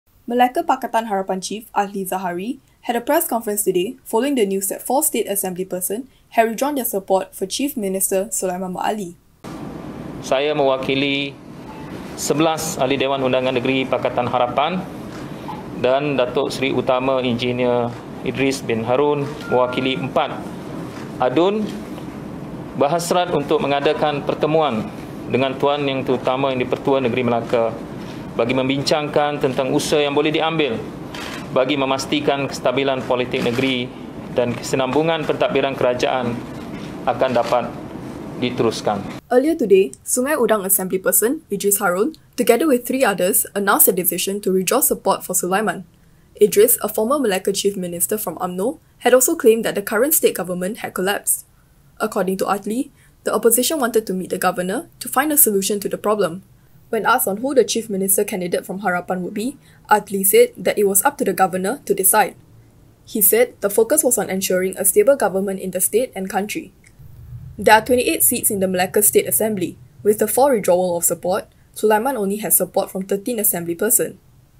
11 Harapan Malacca reps, four others call for meeting with governor
All 11 Pakatan Harapan state assemblypersons in the Malacca state government and four others who had withdrawn support for Chief Minister Sulaiman Md Ali will seek a meeting with Malacca Governor Ali Rustam, said state Harapan chief Adly Zahari.

"The meeting is to discuss efforts that can be taken to ensure political stability in the state and the continuity of the state administration," said Adly, a former Malacca chief minister from Harapan.

This follows a press conference held today, led by Sungai Udang assemblyperson Idris Haron, who, together with three others, announced their decision to withdraw support for Sulaiman.